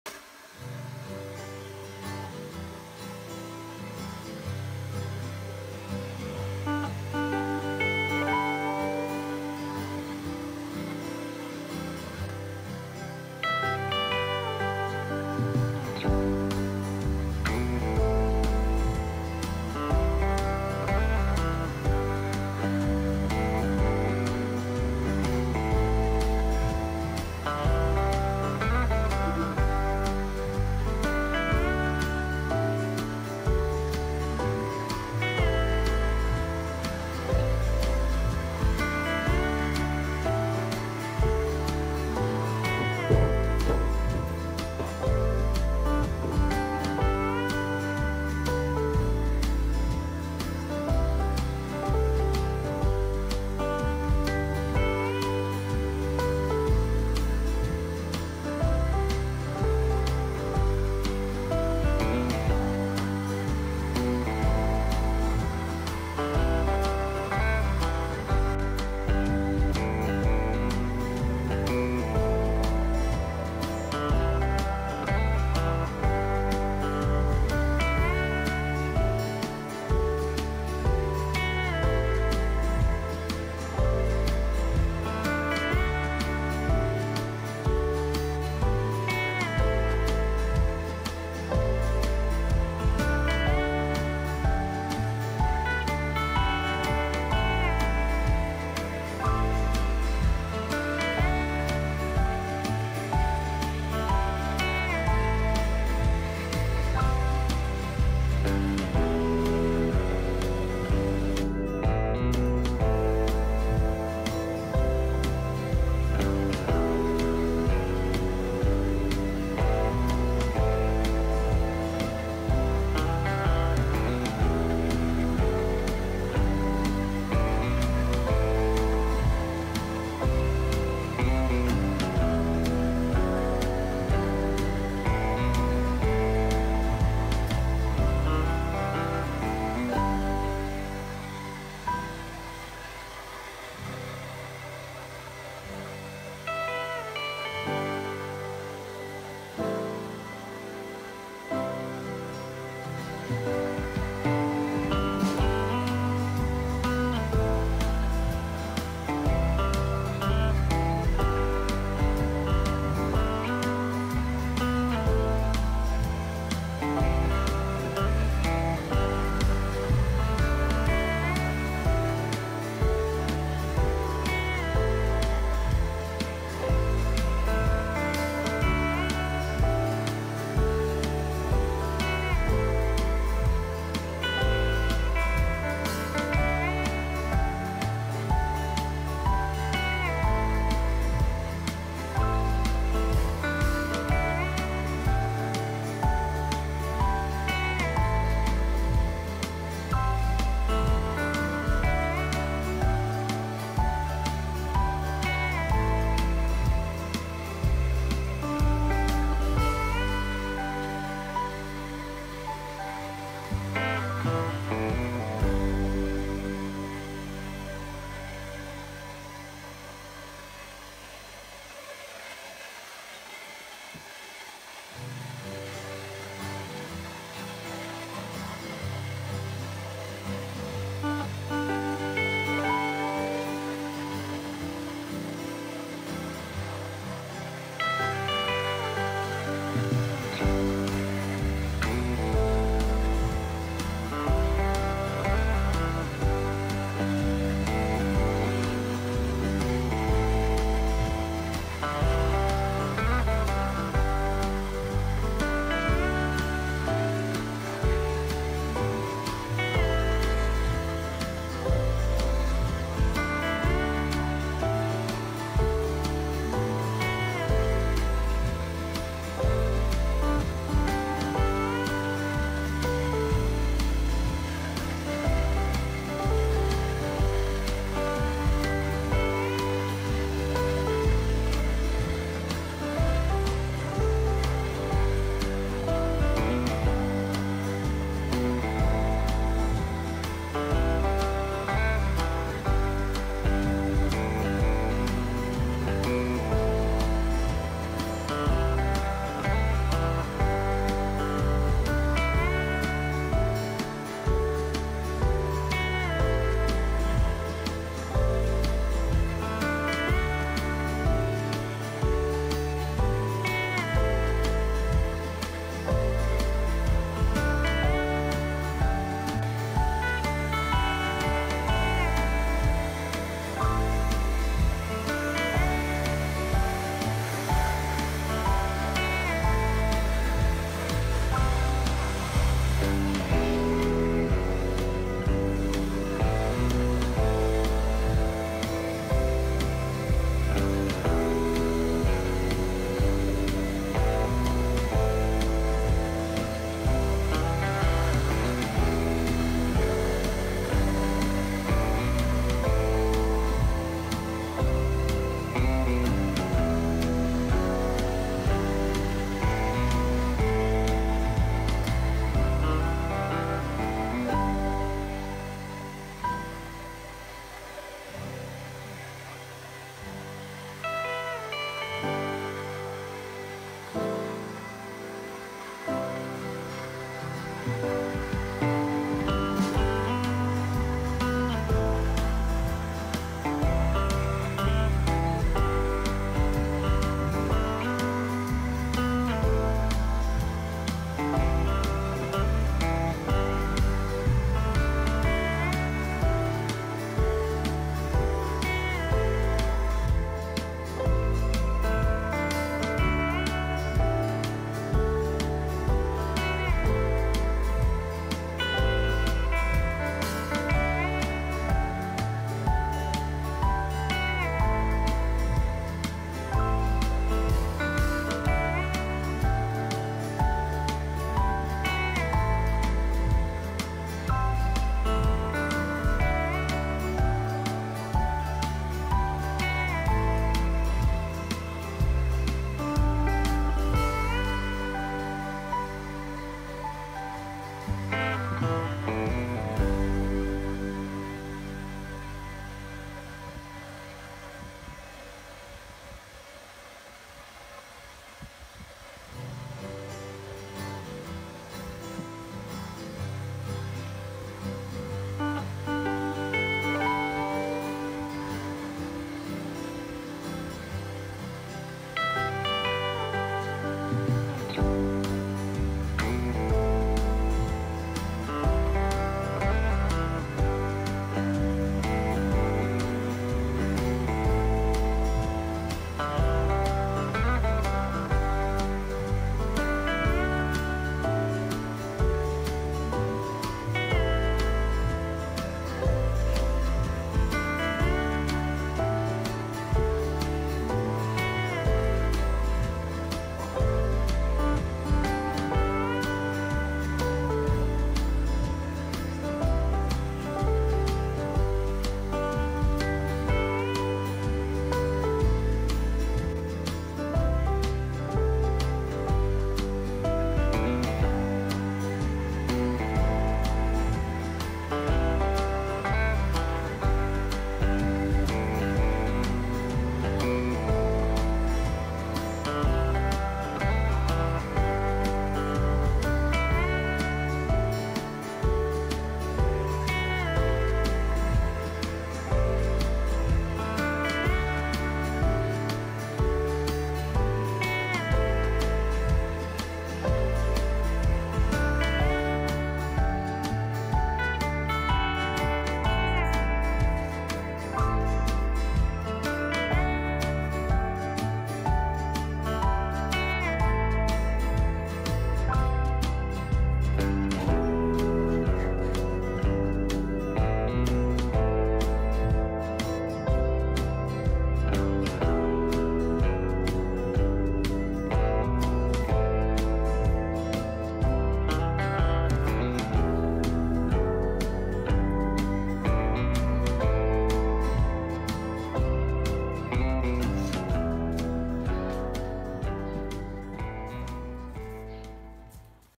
Cute girl blowing hair for LUCKY after shower.
👉Cute girl blowing hair for LUCKY after shower. LUCKY don't like to keep his hair wet.
🙏Thank you so much my goodness people for watching my videos.
💠LUCKY is a disability dog from birth, I hope everyone love and like him too.